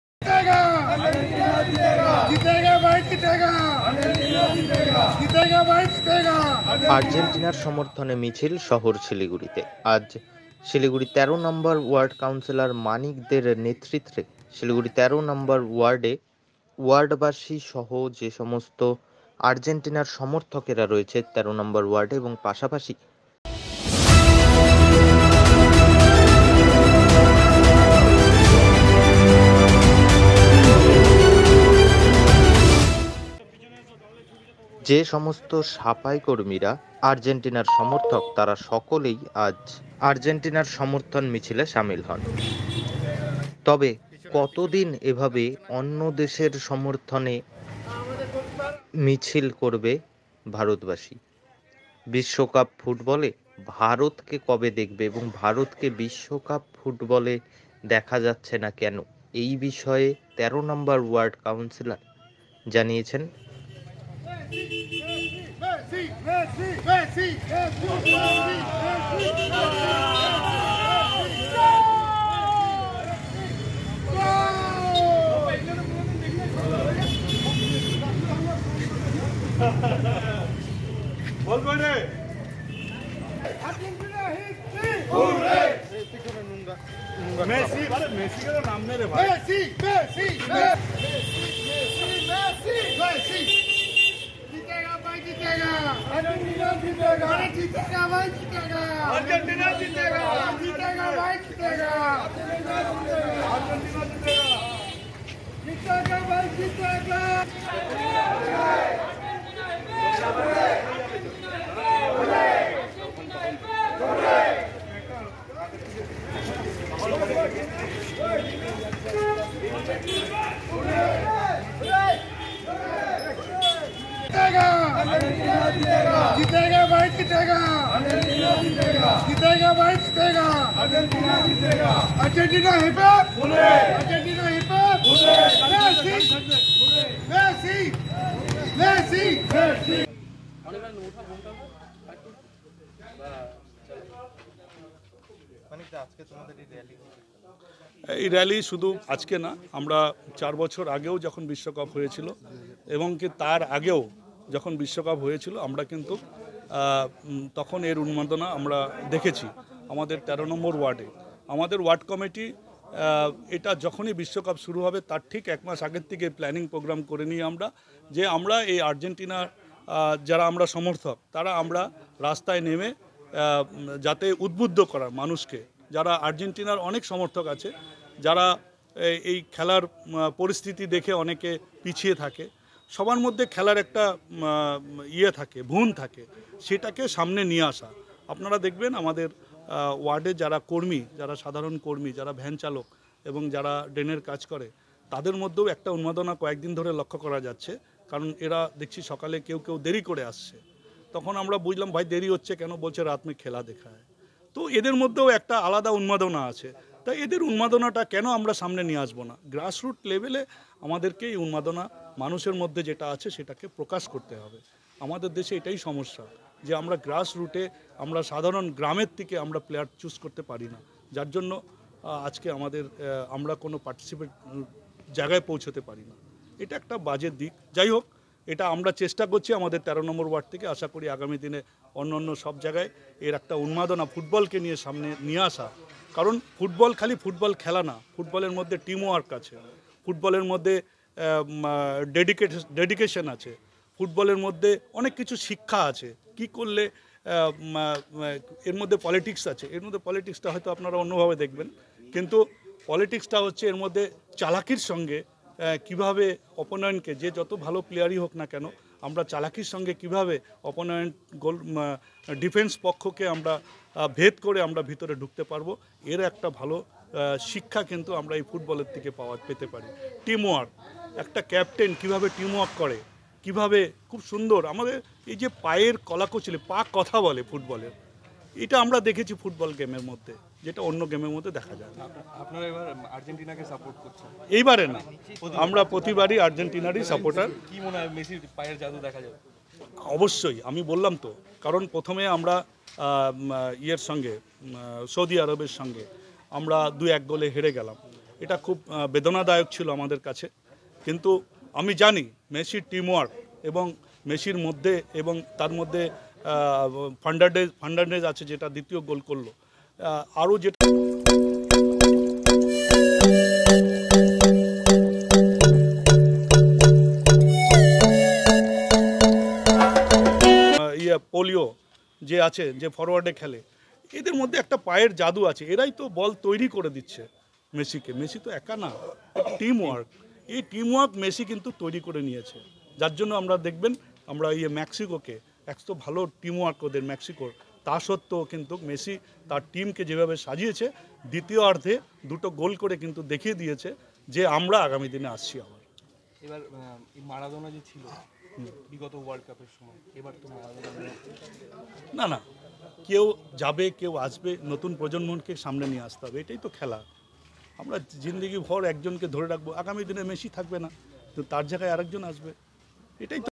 FIFA World Cup : আর্জেন্টিনার সমর্থনে মিছিল শিলিগুড়ি ১৩ নম্বর ওয়ার্ডে
আর্জেন্টিনার সমর্থনে মিছিল শিলিগুড়ি ১৩ নম্বর ওয়ার্ডে